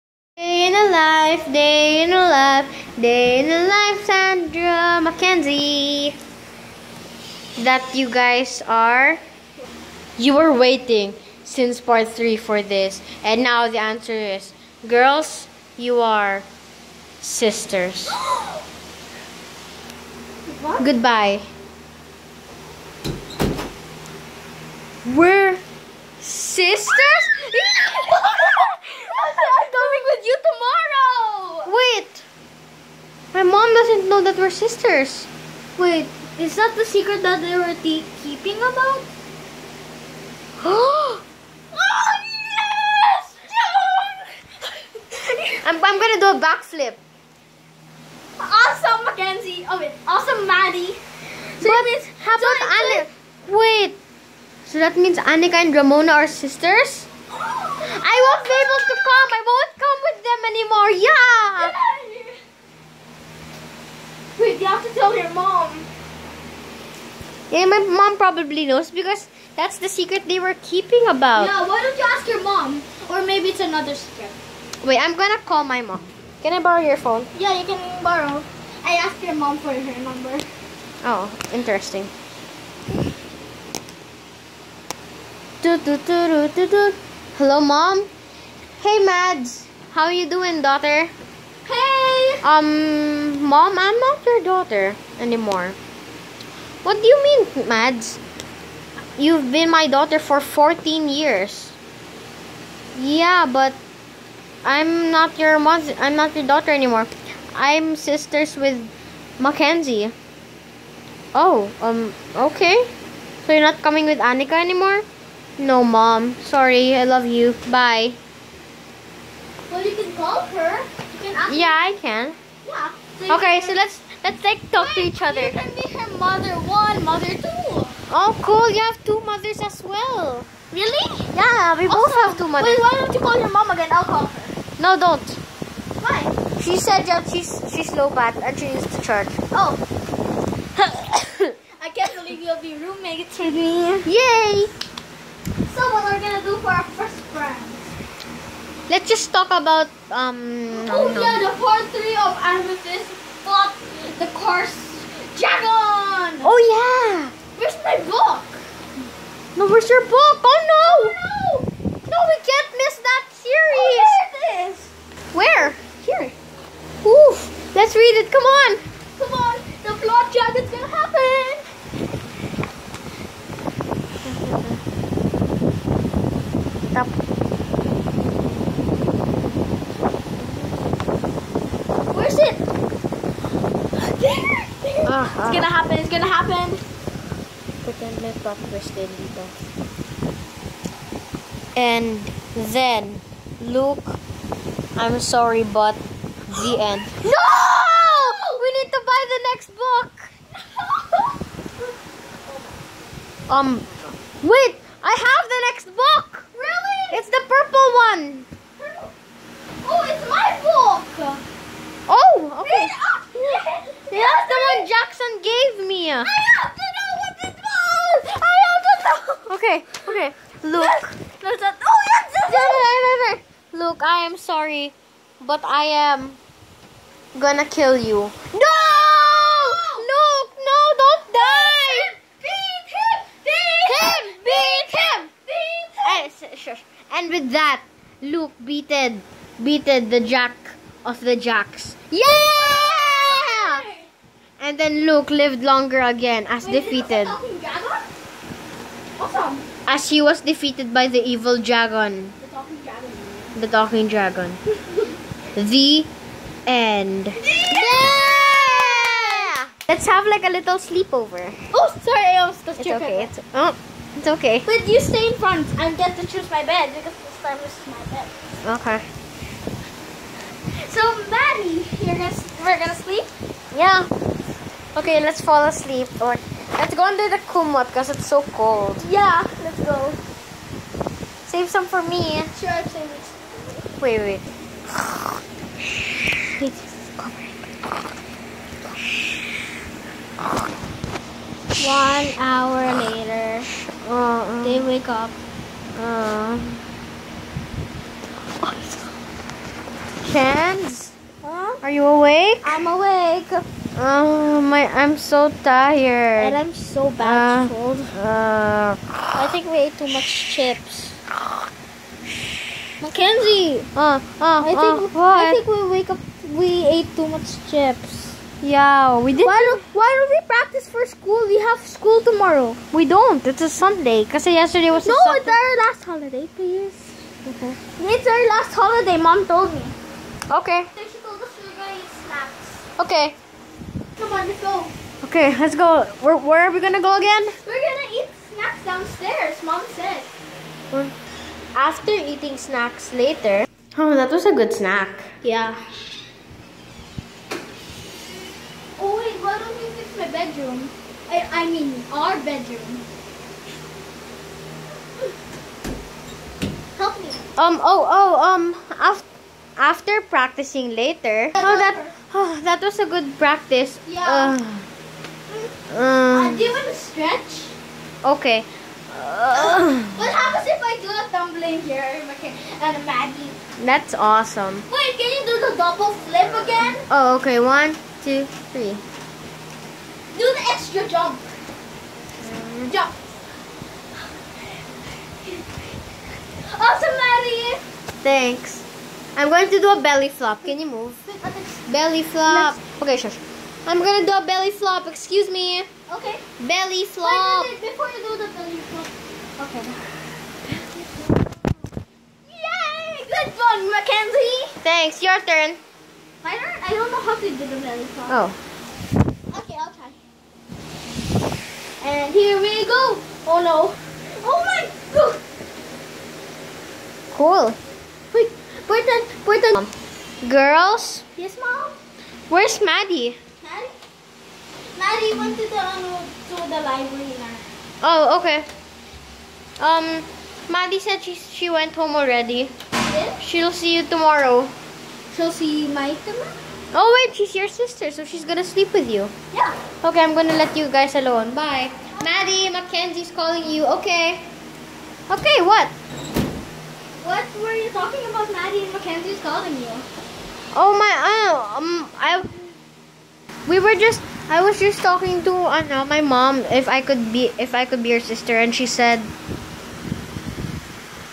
Life as Mackenzie Pt.4
Hey guys! Wow, part 4 already?! Time fliezzz...Today me and sandy decided to make a movie/series of Life As Mackenzie. Mackenzie is a normal girl with big dreams and a special life. Watch the whole series to find out! Btw, its free to watch! :)

CAST OF CHARACTERS:
Alex:Mom,Maddie,Teacher,Ramona,Jackson, Layla

Sandy:Mackenzie Lee Aurdura